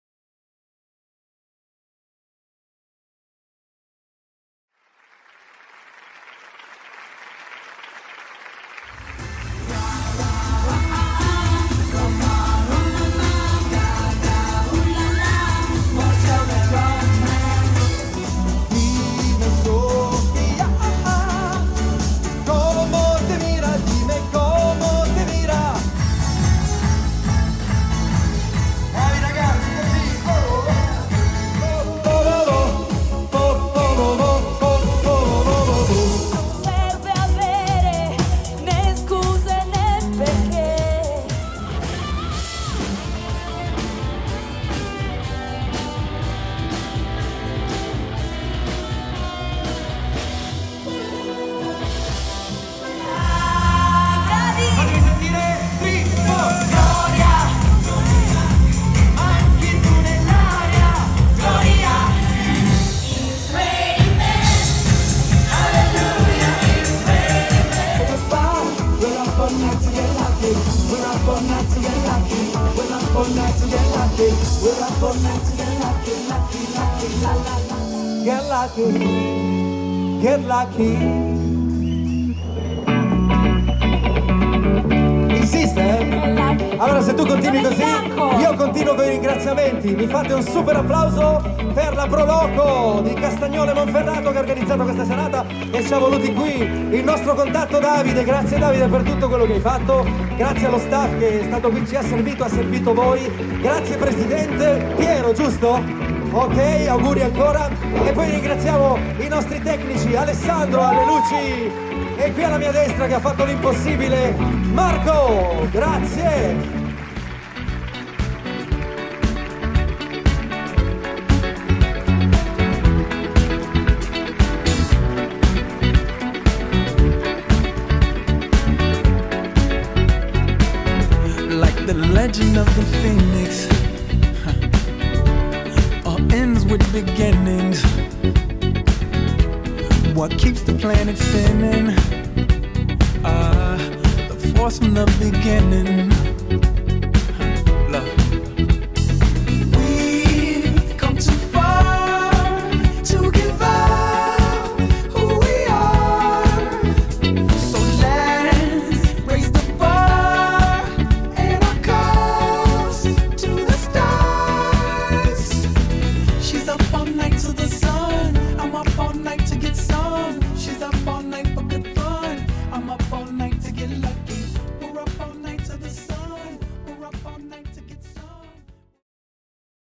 Live Disco Show OniOn a Castagnole Monferrato
"Daniele & the OniOn Rings" Live per la Festa di Sant'Anna a Castagnole Monferrato